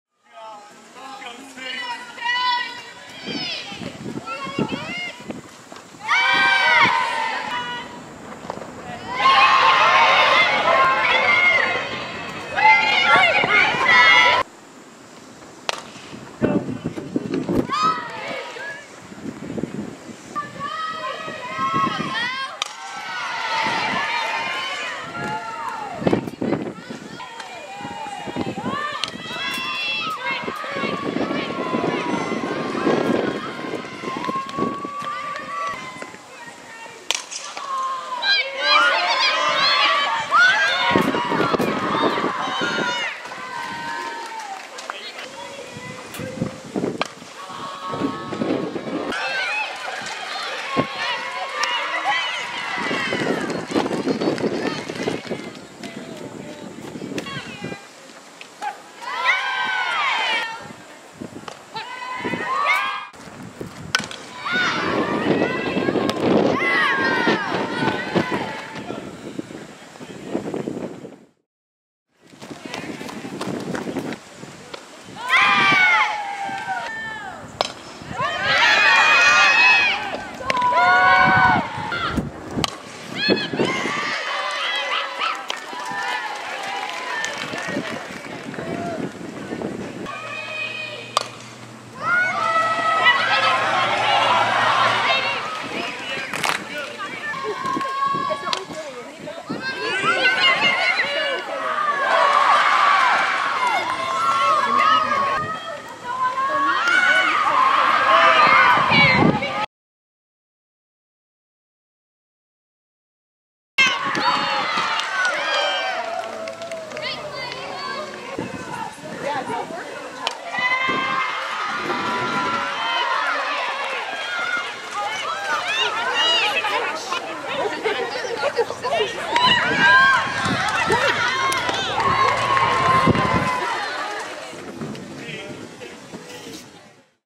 Willard Tiger Softball
Tiger highlights from their Jamboree game with Glendale.